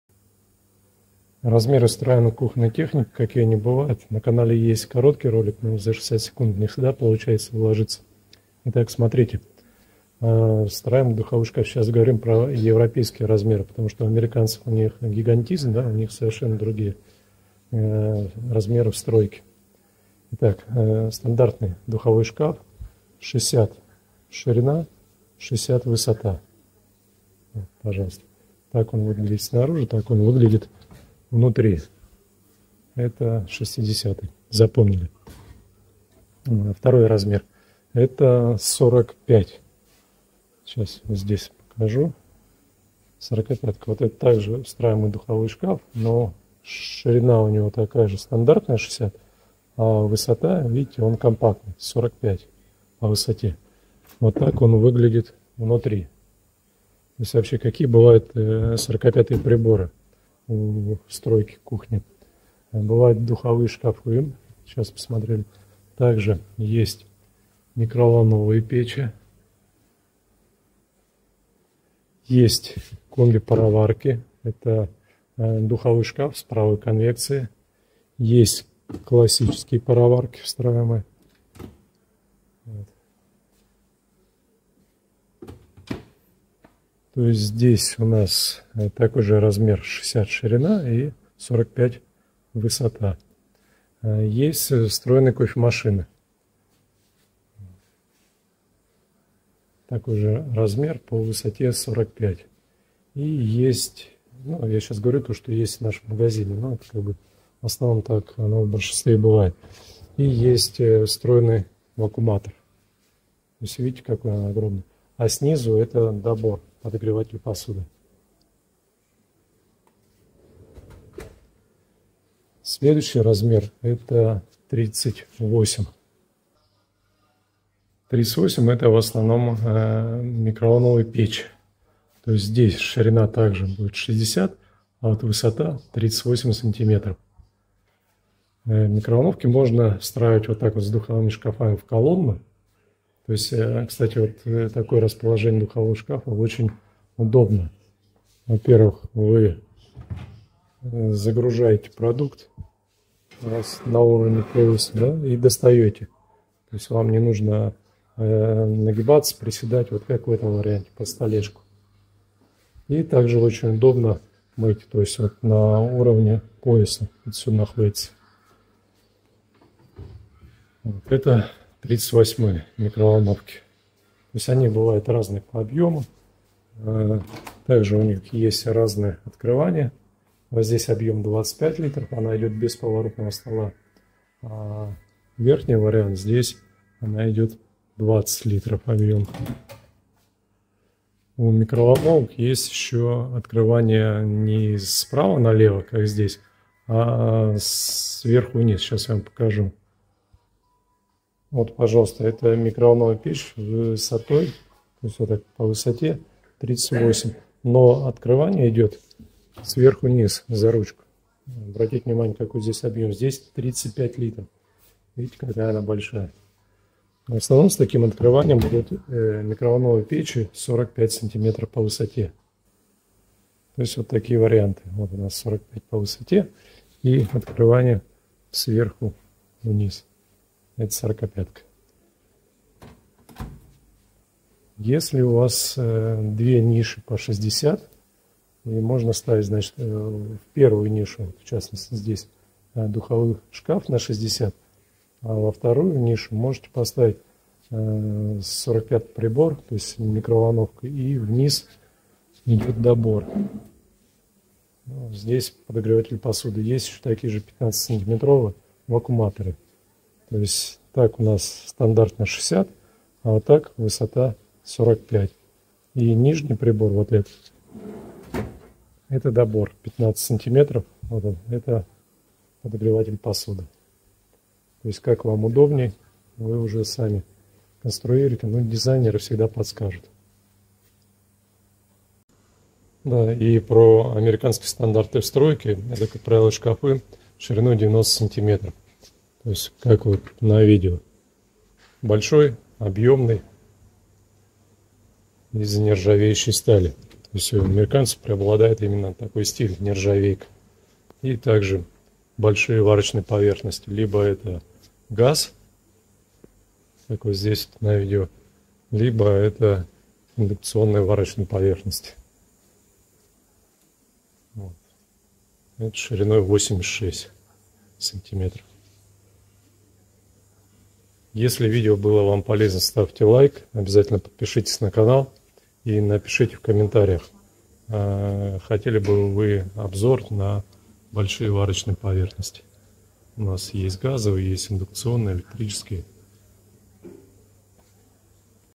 Встраиваемая Бытовая Техника и Размеры для Встройки в Кухню
Какие бывают размеры встраиваемой бытовой техники Европейские и Американские стандарты встройки.

● Пишите комментарии 💬
● Чтобы не пропустить новые ролики на канале ,  жмите 🔔